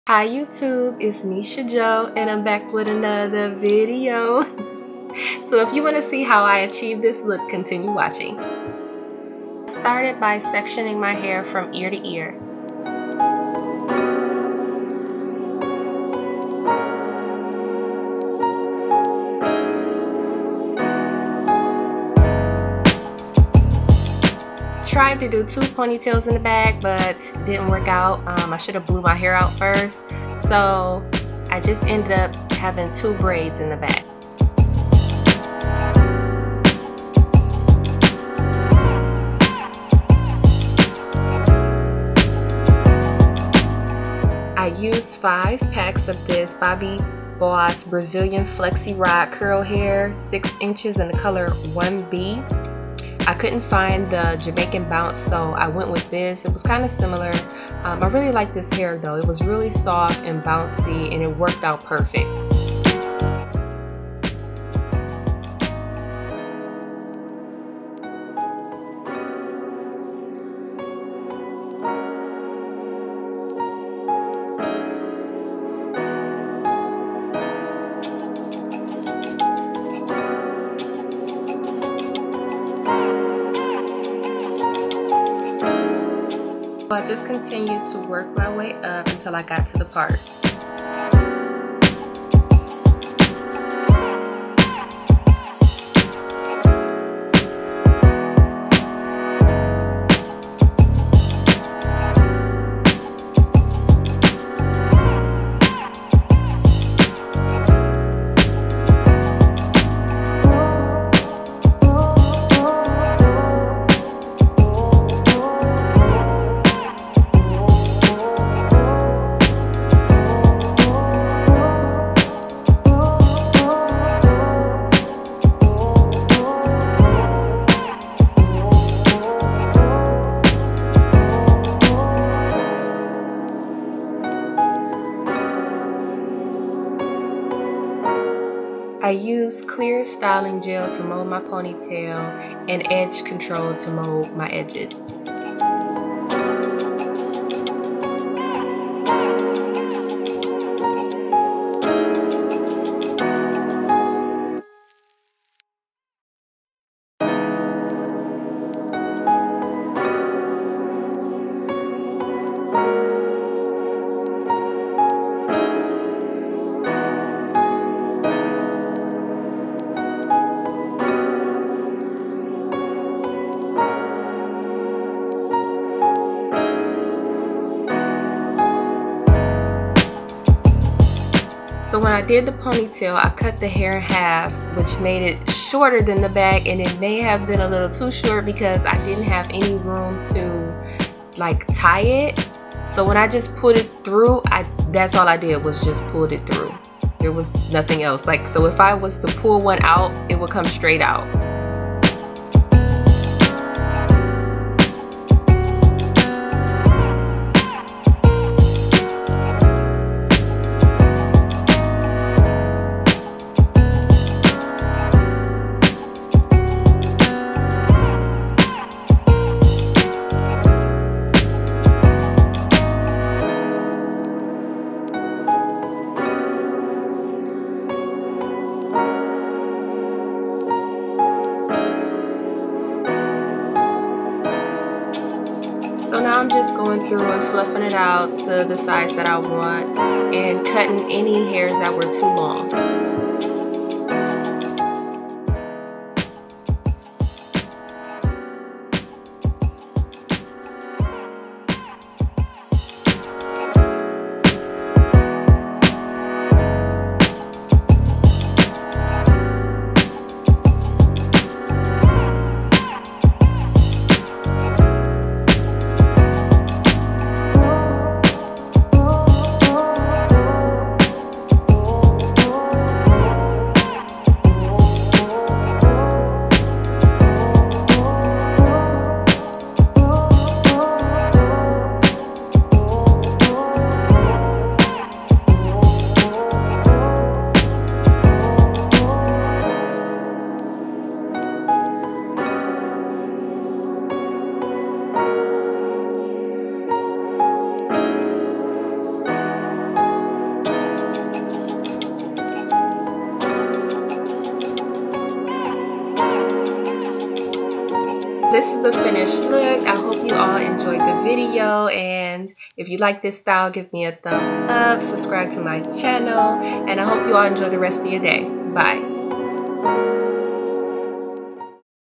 Half Up HAlf Down Curly Hair Crochet
HI GUYS! THE HAIR I USED FOR THIS STYLE IS BOBBI BOSS BRAZILIAN FLEXIROD CURL6" IN THE COLOR 1B, 5 PACKS. LET ME KNOW WHAT YOU THINK.